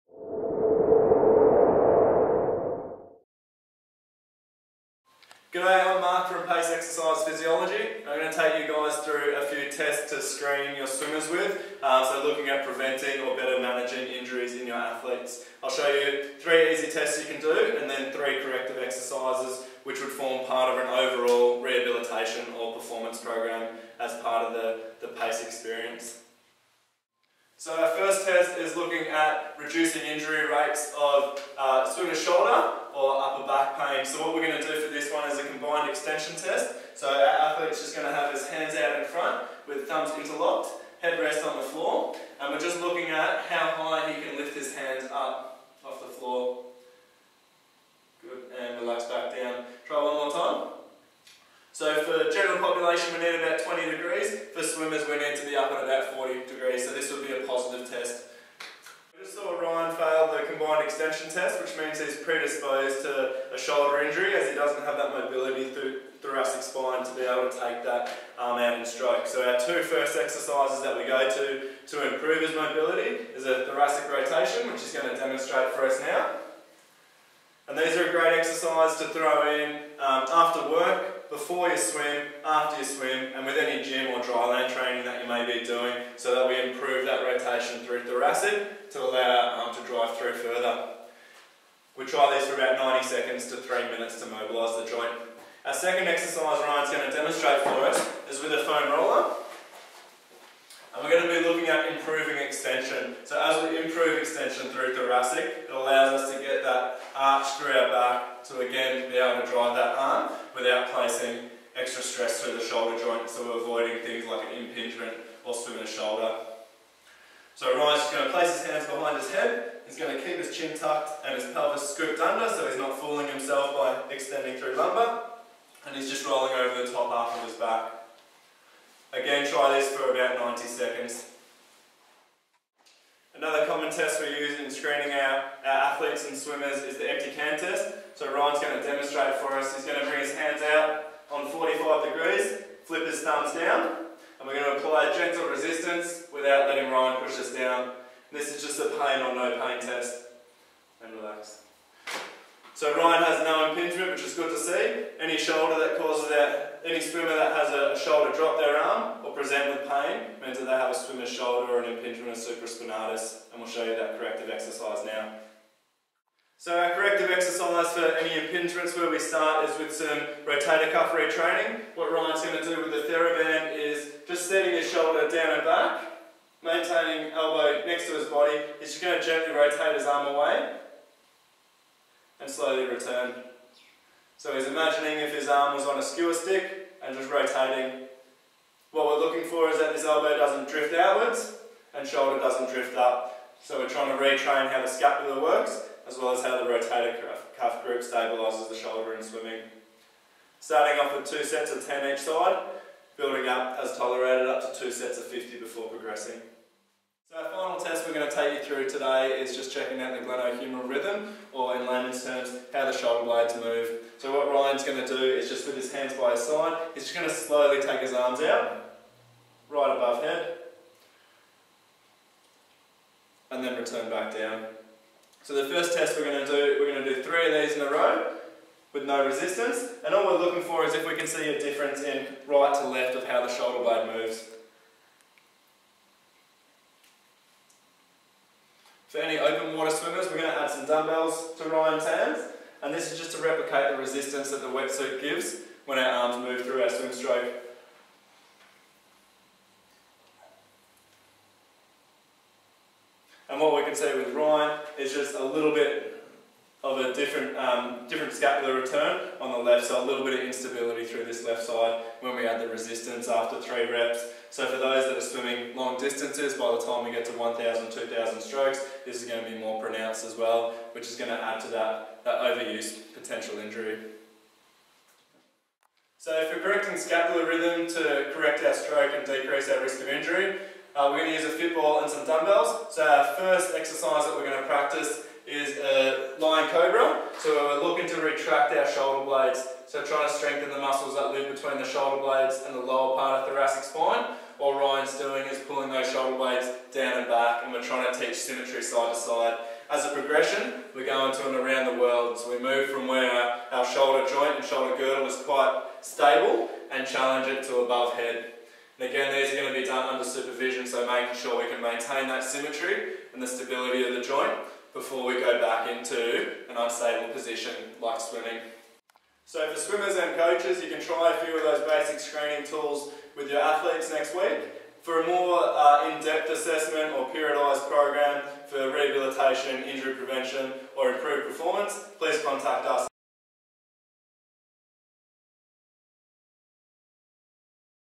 Swimming Injury Prevention
Senior Exercise Physiologist, Mark Simpson, takes you through 3 musculo-skeletal screening tests to check your swimmers for most common swimming injuries or muscle imbalances, followed by top 3 corrective exercises to decrease injury risk or rehabilitate to keep your athletes swimming more efficient and injury free.